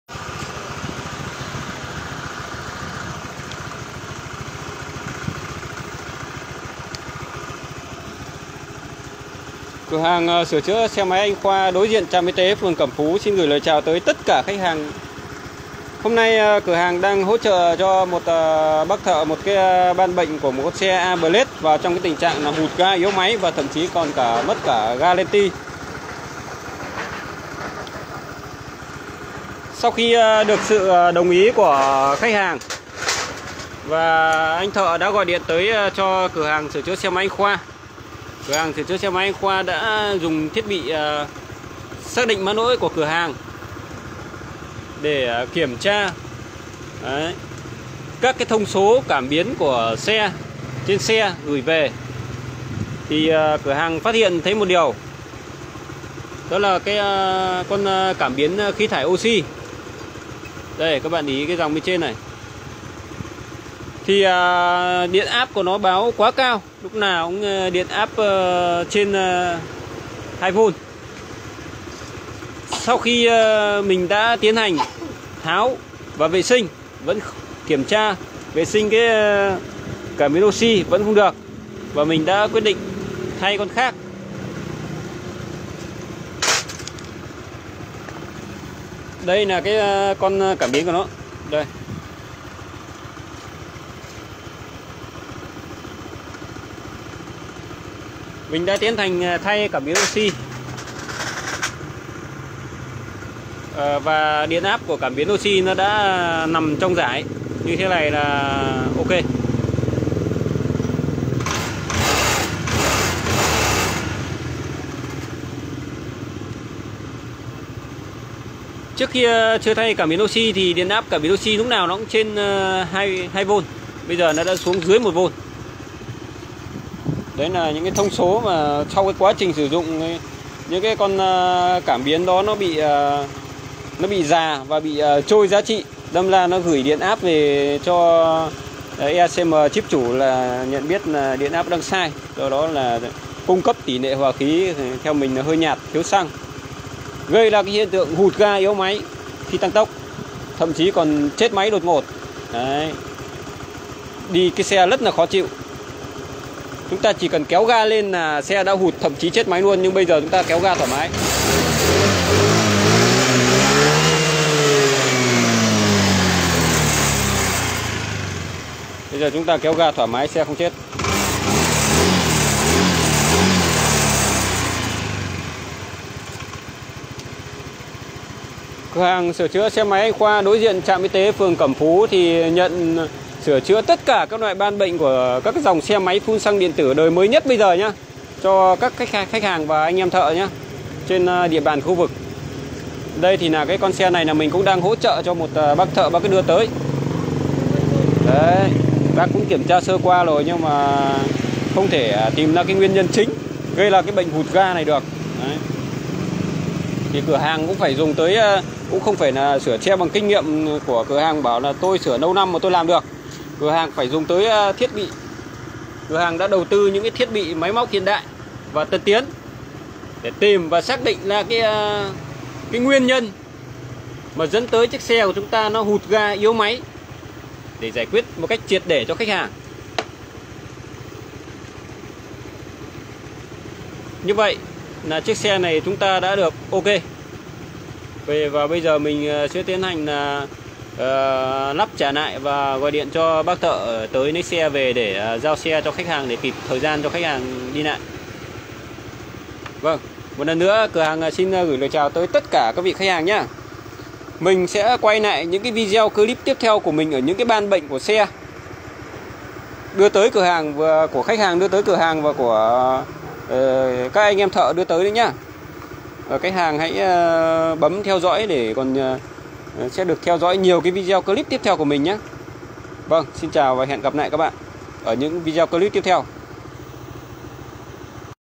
Anh Khoa: Xe AB hụt ga yếu máy do cảm biến O2 báo sai
Nguồn: Anh Khoa